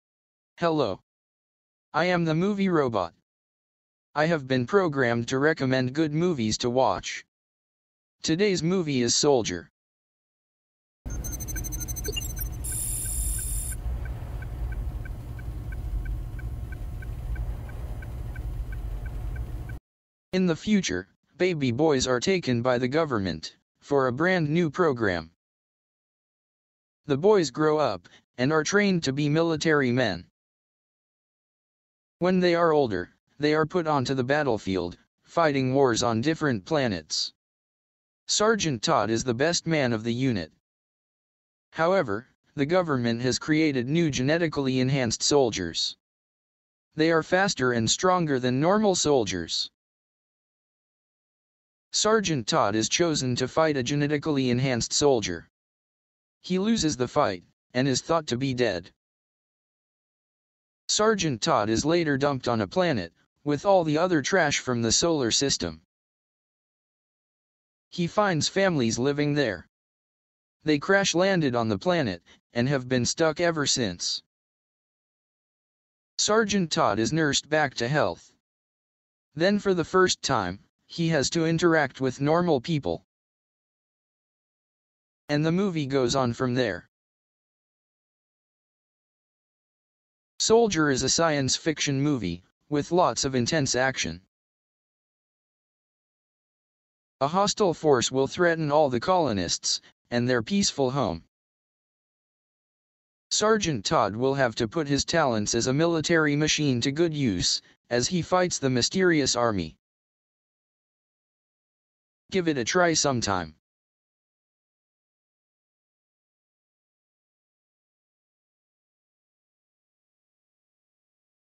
Movie Robot suggests Soldier (1998)
Here's a brief description of this science fiction movie.  This video is dedicated to Gary's Wizard Beard.  Thanks for reminding my programmers how good this movie is.